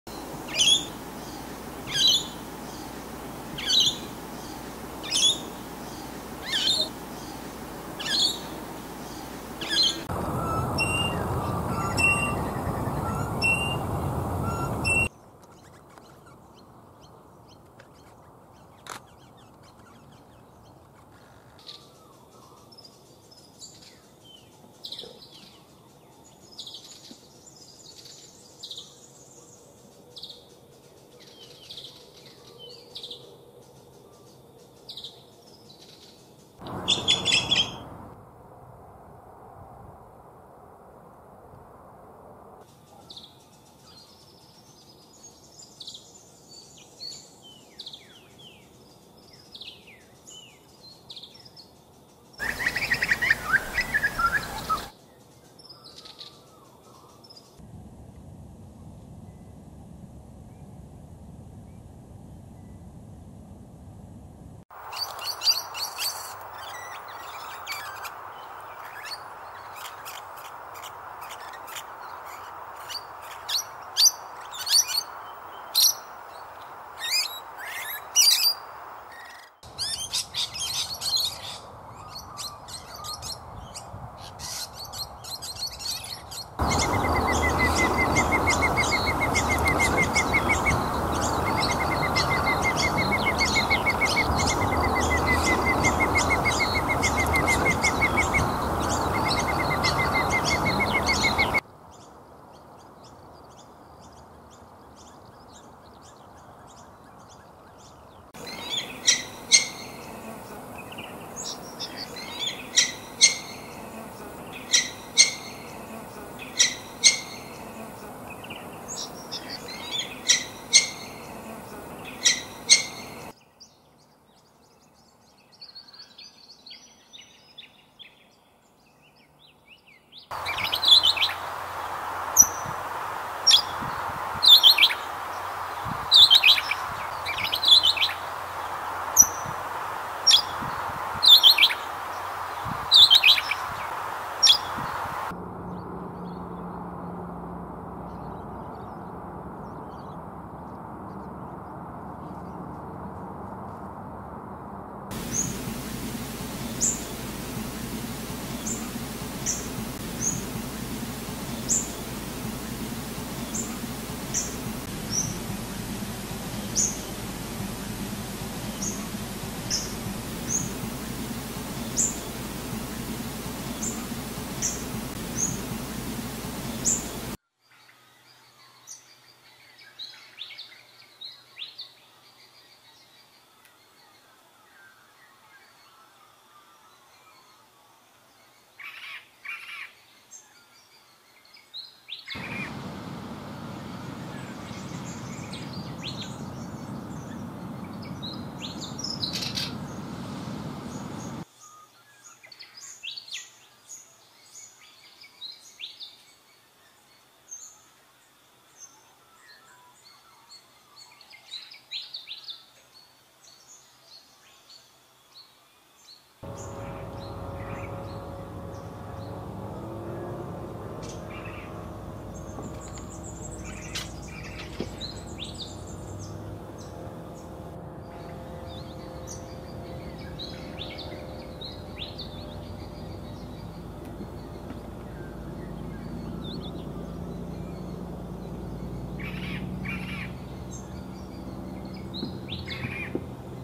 Birds of Victoria - Part VIII
* Birds of Victoria - Part VIII  - There is a large amount of birds living in this state, here is a collection of some of them with images, video with some information of our wonderful birds that I have seen, photographed and recorded. I feel very fortunate to have such a variety of habitats to view the many birds that live here, I will add more videos over time, hope you enjoy my work as I enjoy putting them together.

This video includes the: Eastern Rosella "Platycercus eximius", Grey Currawong "Strepera versicolor", Grey Butcherbird "Cracticus torquatis", Musk Lorikeet "Glossopsitta concinna", Pallid cuckoo "Cacomantis pallidus", Peregrine falcon "Falco peregrinus", Red-rumped Parrot "Psephotus haematonotus", Silvereye "Zosterops lateralis", Singing Honeyeater "Lichenostomus virescens", and the Straw-necked Ibis "Threskiornis spinicollis".

Locations are all over the place within Victoria, Australia